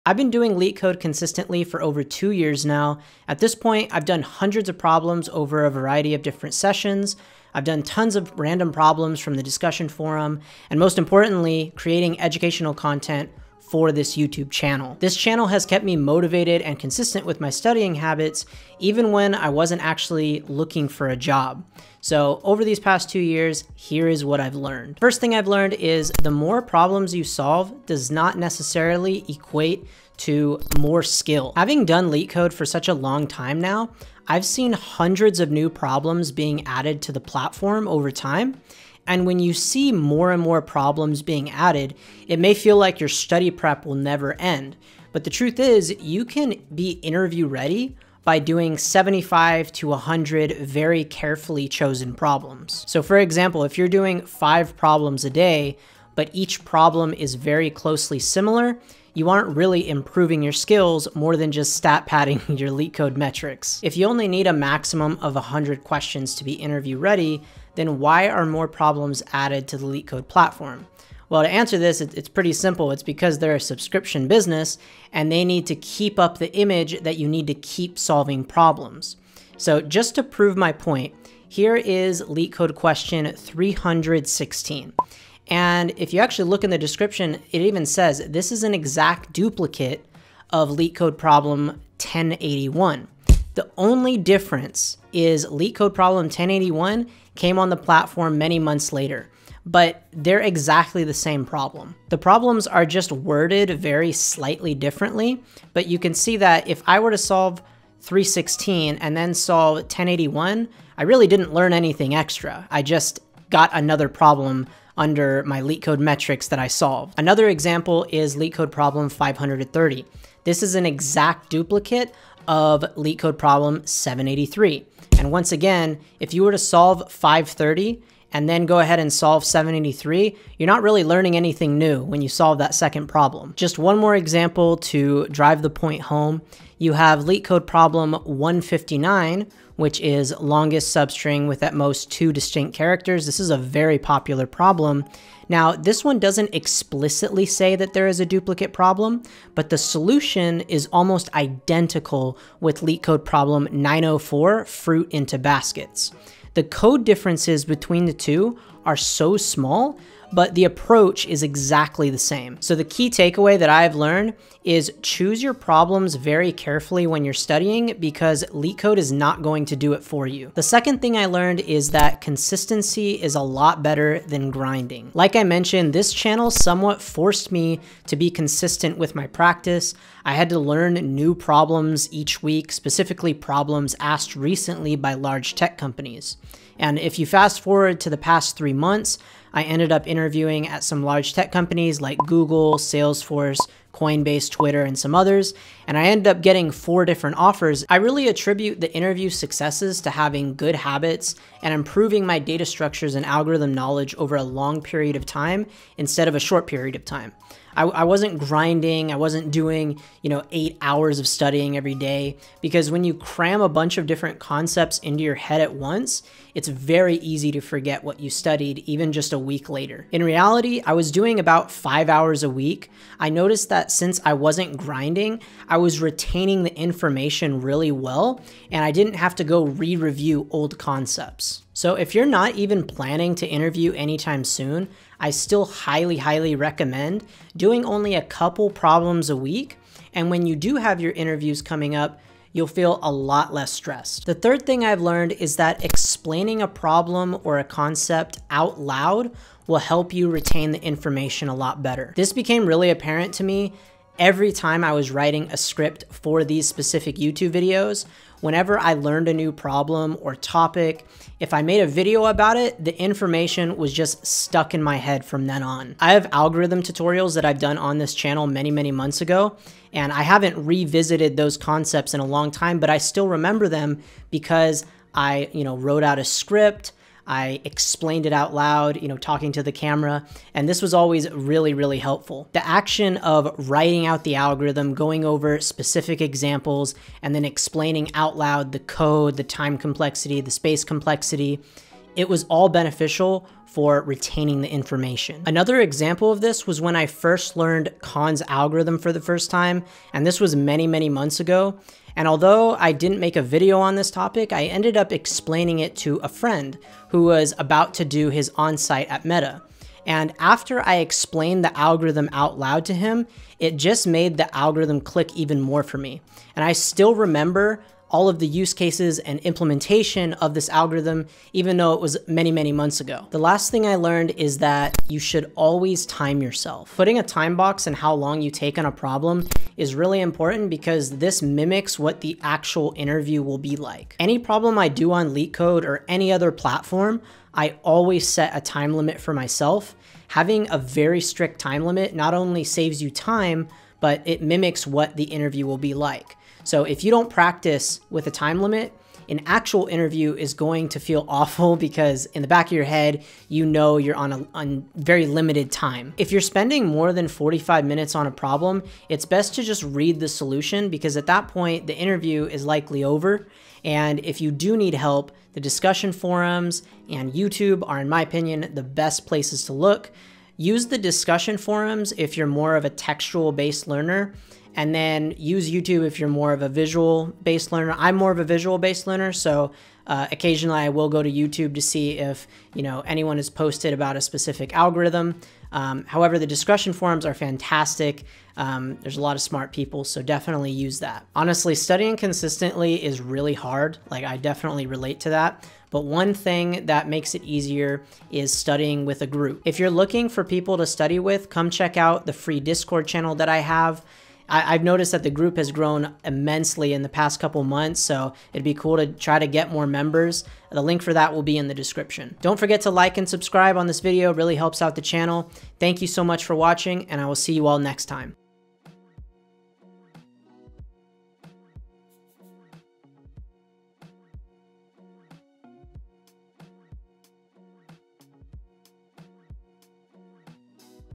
How to LeetCode effectively in 2022
Knowing the proper way to "LeetCode" is a skill in itself. I have been doing LeetCode problems consistently for over two years and here is what I have learned.

First, higher problems solved does not necessarily equate to better skill. You can be interview ready for any tech company with about 100 carefully chosen problems. Choose your problems wisely because LeetCode will not do it for you. Second, consistency is better than grinding. Even if you are not planning to interview soon, start prepping now. Third, explaining a concept out loud will help you retain the information better. Lastly, always time yourself. In an interview, you will always be under a time limit and it is best to prepare in the same way.